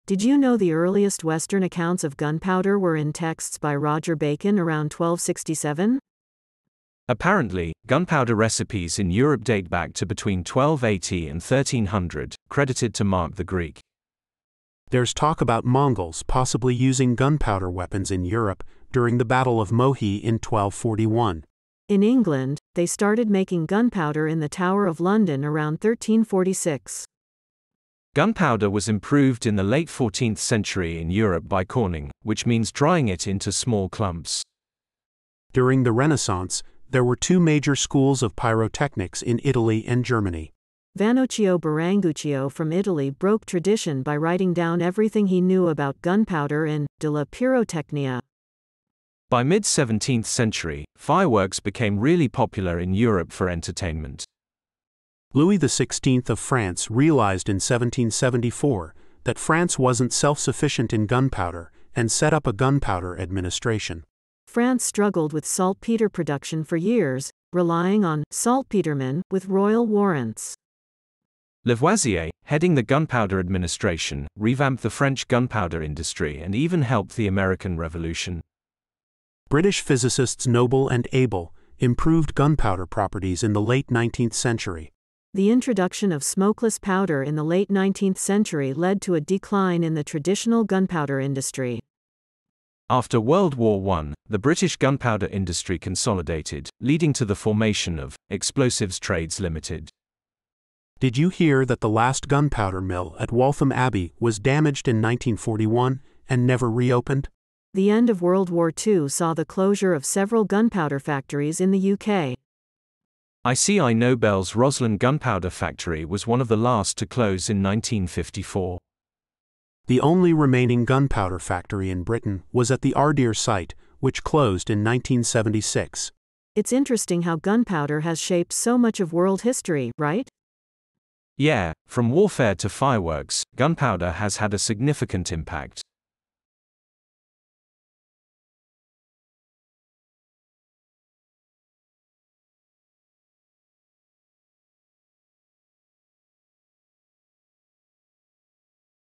GUNPOWDER IN EUROPE
The article discusses the history of gunpowder in Europe. It begins with early accounts by Roger Bacon in 1267 and includes recipes dating back to the 1280s. There's mention of possible Mongol use of gunpowder in Europe in 1241. In England, gunpowder production began in 1346, and by the late 14th century, gunpowder was improved by corning. During the Renaissance, significant pyrotechnic advancements were made in Italy and Germany, notably by Vannoccio Biringuccio. By the mid-17th century, fireworks became widespread in Europe. In the 18th century, France, under Louis XVI and Antoine Lavoisier, revamped its gunpowder industry, impacting the American Revolution. In the late 19th century, British physicists improved gunpowder, but its industry declined with the introduction of smokeless powder. Post-World War I, the British gunpowder industry consolidated, leading to closures of many factories. The last gunpowder factory in Britain closed in 1976. The article highlights gunpowder's significant impact on history, from warfare to entertainment.